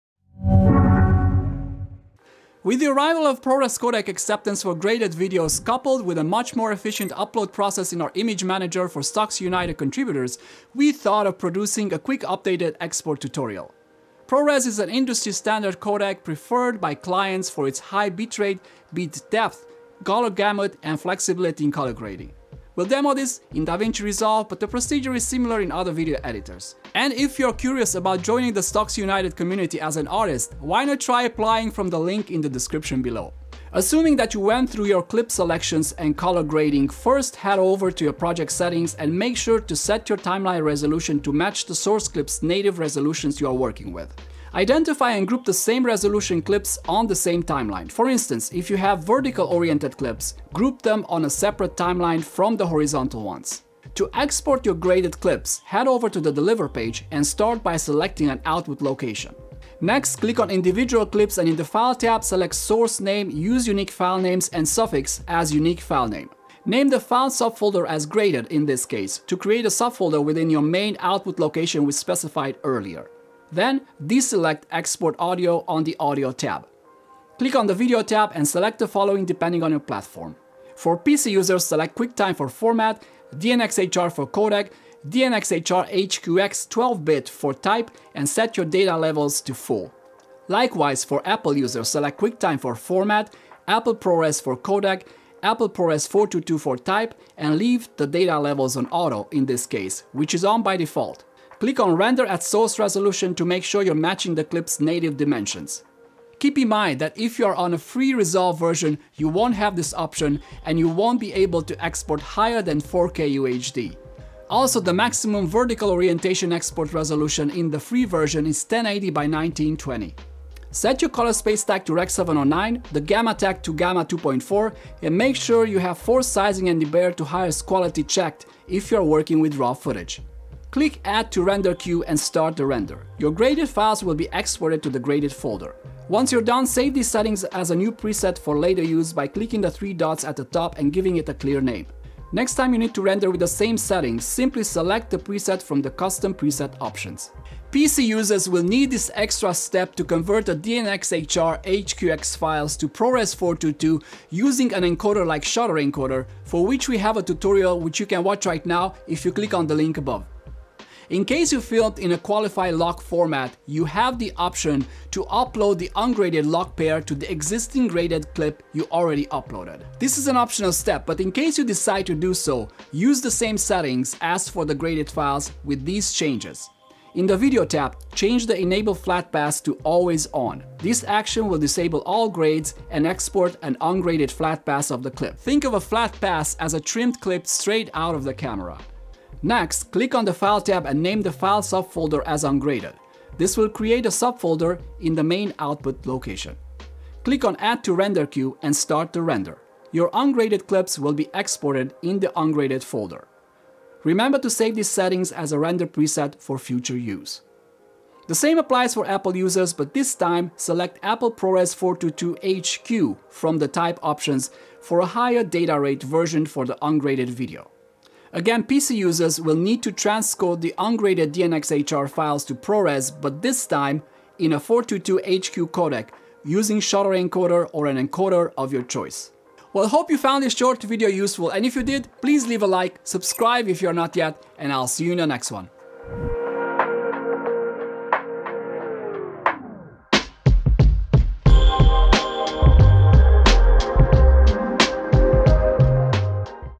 Updated Video Export Settings for Stocksy United
With our latest rollout for accepting ProRes files, we thought it helpful for Stocksy United contributors to go through the latest updated video export settings. ProRes is an industry-standard codec, popular in the professional post-production community because of its flexibility in editing, broad platform support, color grading performance, high-quality compression, and bit rate.
Are you looking to start generating revenue with your footage? Apply to join Stocksy United as an artist